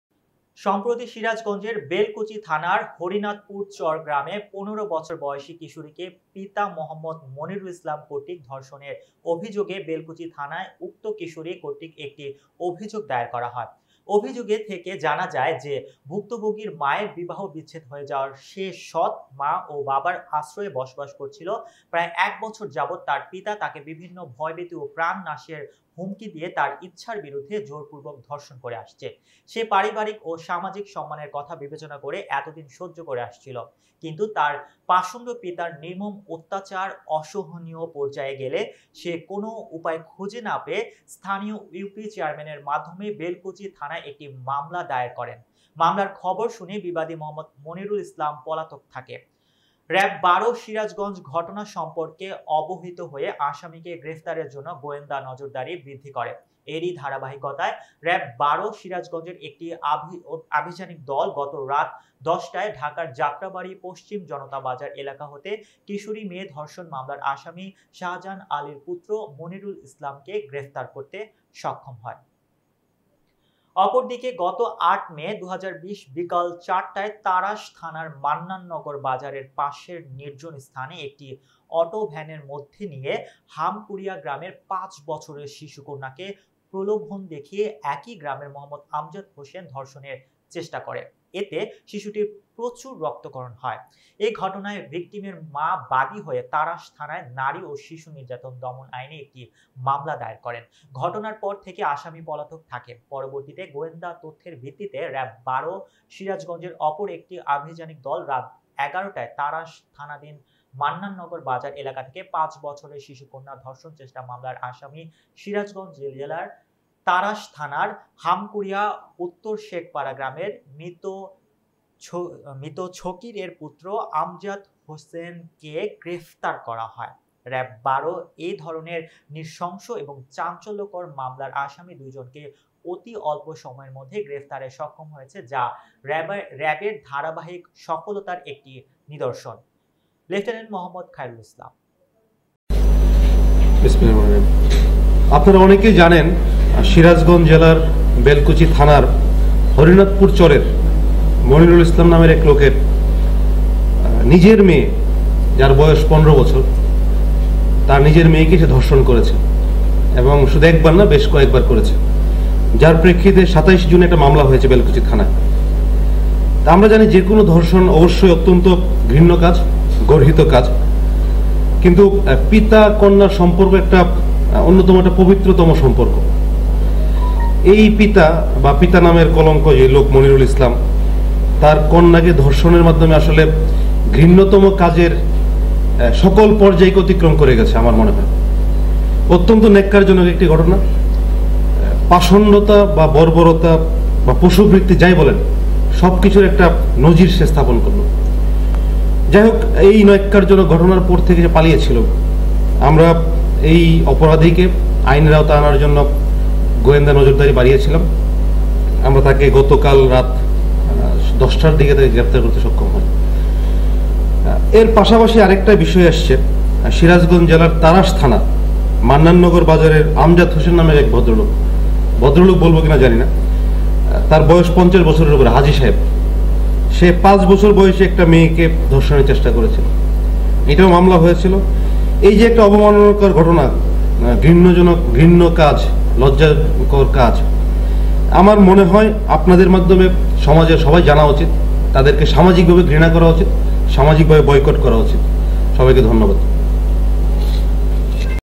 সিরাজগঞ্জ র‌্যাব-১২ এর বিশেষ অভিযানেকিশোরী কন্যা ধর্ষণের দায়ে পিতা এবং
সিরাজগঞ্জ র‌্যাব-১২ এর বিশেষ অভিযানে
কিশোরী কন্যা ধর্ষণের দায়ে পিতা এবং